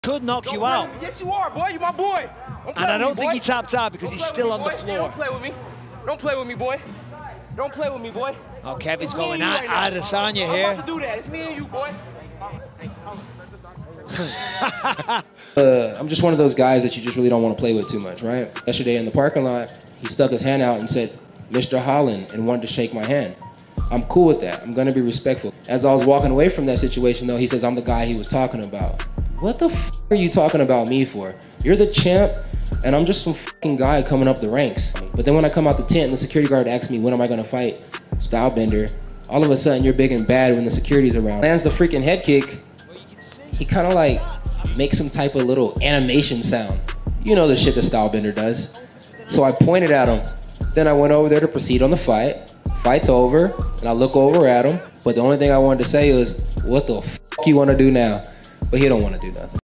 When Kevin Holland trash talked Israel Adesanya right after his fight…😂
Kevin Holland trash talks Israel Adesanya right after his fight and says why he’s doesn’t like him…